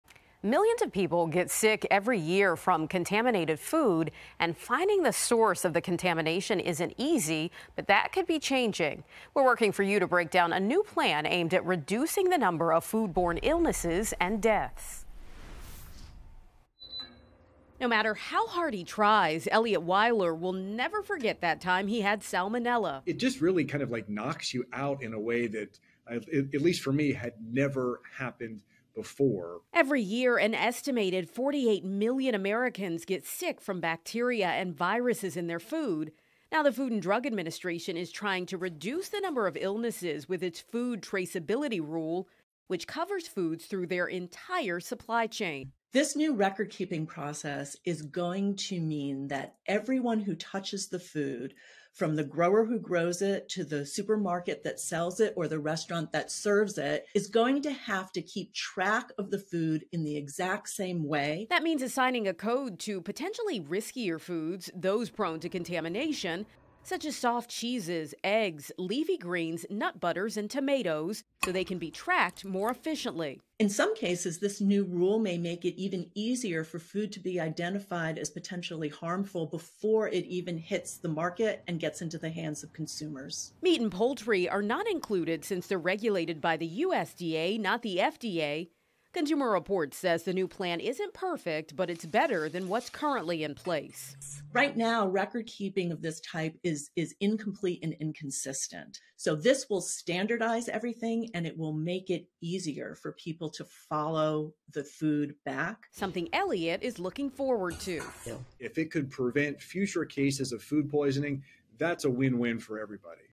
New FDA rule on food poisoning
Millions of people get sick every year from contaminated food. Finding the source of the contamination isn't always easy, but that could be changing.